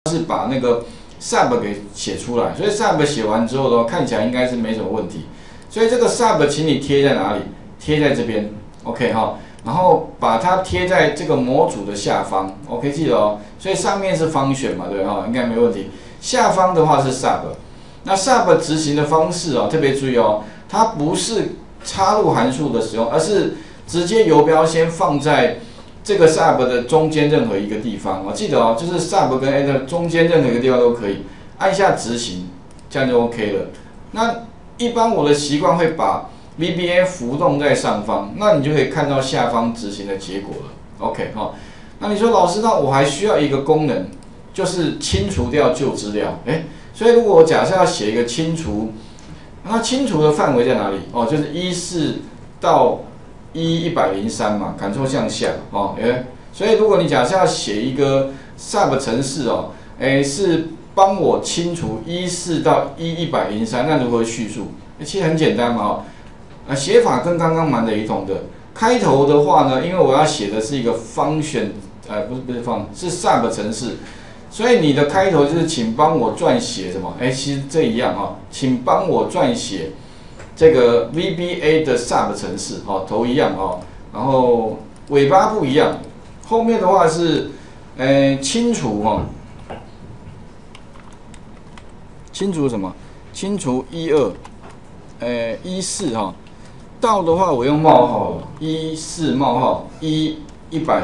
04 用ChatGPT產生清除的SUB程式
台師大試算表進階應用與程式設計202401B班第3次上課

上課內容：
01_重點複習與利用ChatGPT產生VBA的Function程式
02_用ChatGPT產生VBA的Function程式
03_用ChatGPT產生VBA的SUB程式
04_用ChatGPT產生清除的SUB程式
05_新增兩個按鈕與程式說明與啟用巨集

課程理念：
1.以循序漸進的方式, 透過詳細的說明和實用的50個Excel VBA範例,
2.進而解說EXCEL與資料庫的結合，甚至將EXCEL當成資料庫來使用，

吳老師  113/9/20